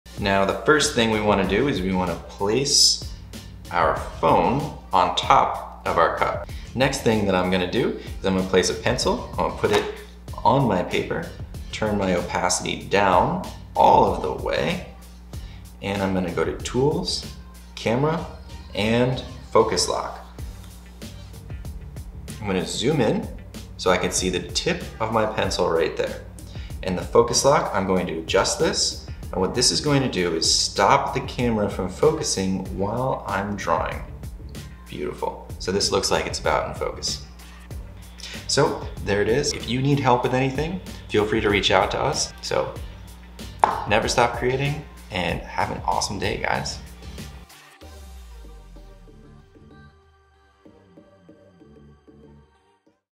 Pro Tip: How to stop the camera from focusing while you're drawing
Download Da Vinci Eye!

Classic Mode Phone Stand:

AR Mode Phone Stand: